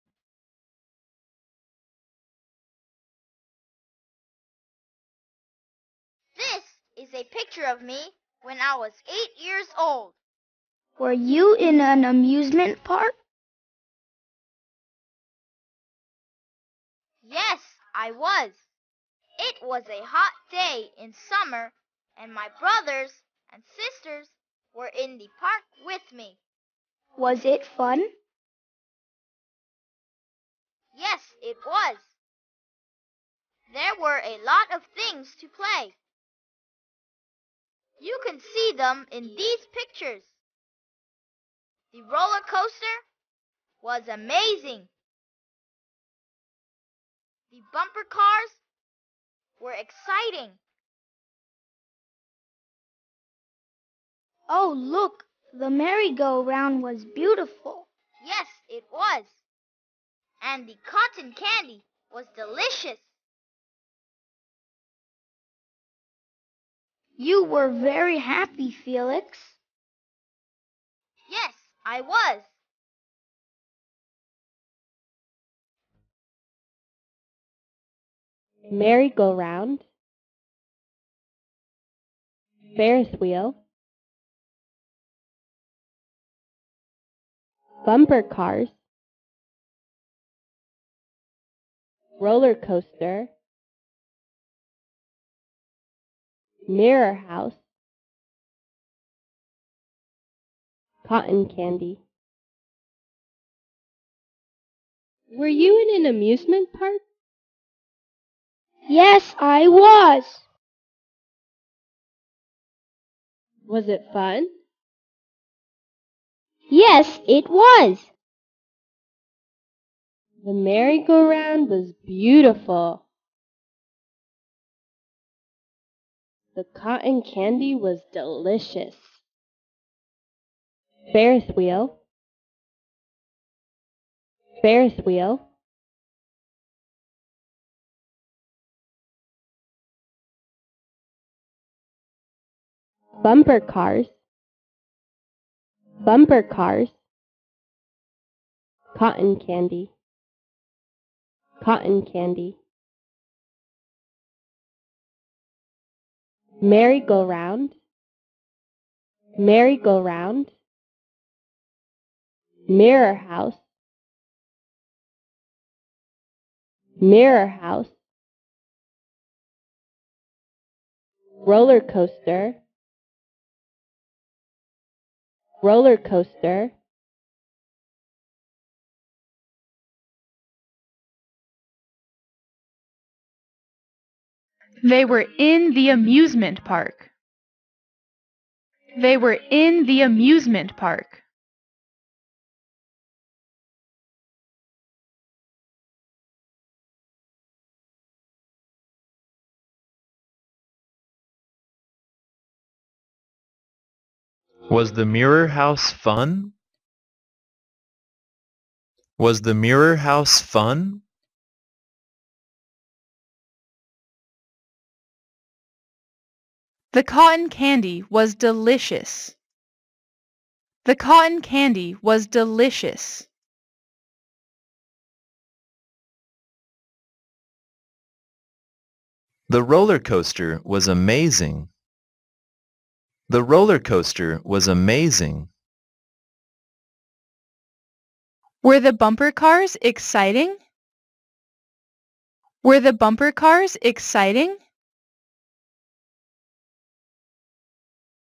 Hello Felix | CVL Unit 8 The amusement - Learning english for kids
Hello Felix | Conversation & Vocabulary & Listening
 (CVL) Unit 8 The amusement - Learning english for kids

Preparing definions of objects in the amusement park, unit 8 support 10 years old kids in expanding their vocabulary.

You are watching video of CVL Unit 8 The amusement on Hello Felix  - online English teaching channel for children on Youtube !!!